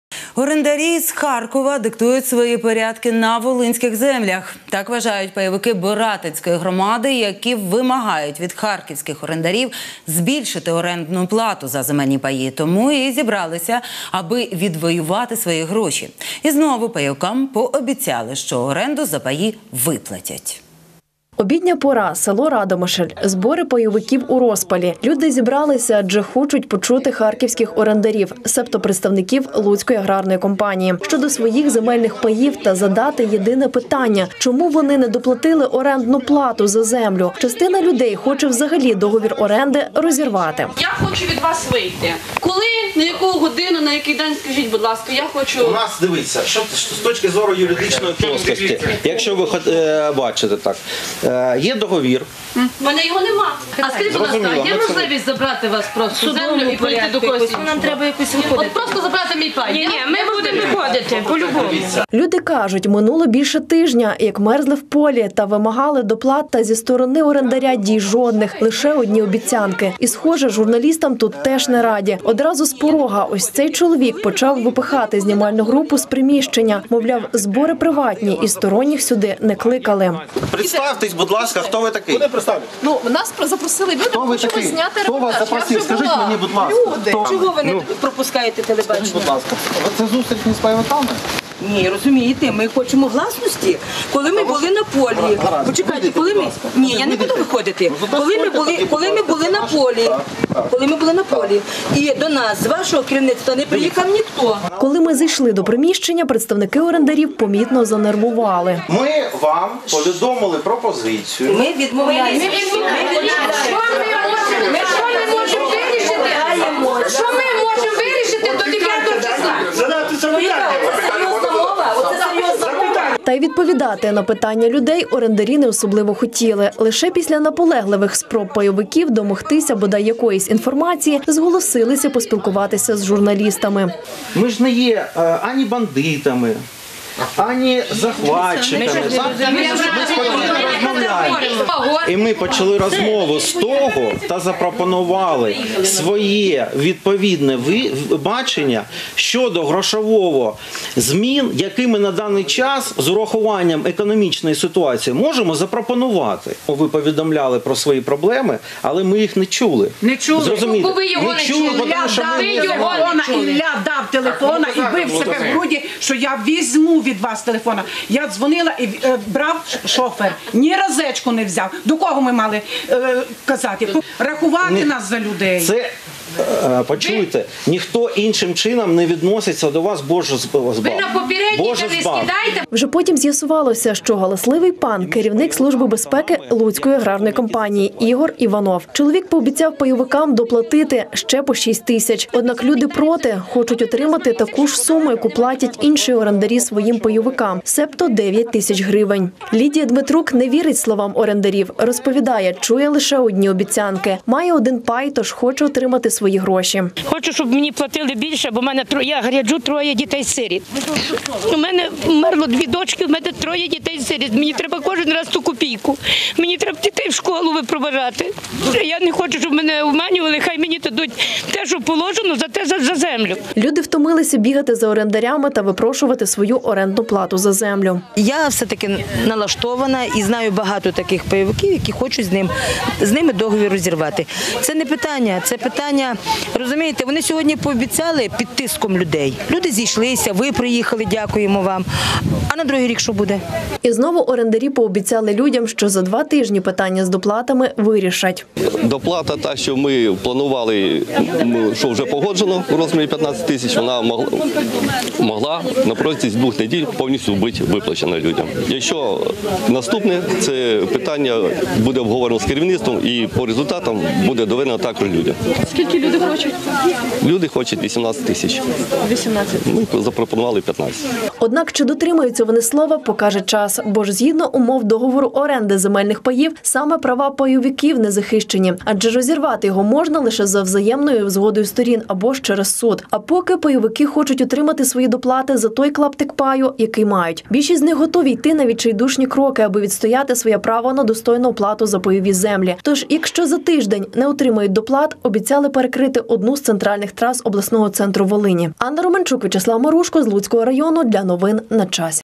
Як на Волині пайовики воюють з харківськими орендарями
Орендарі з Харкова диктують свої порядки на волинських землях – так вважають пайовики Боратинської громади, які вимагають від харківських орендарів, збільшити орендну плату за земельні паї.  Тому й зібралися, аби відвоювати свої гроші.  І знову пайовикам пообіцяли, що оренду за паї виплатять.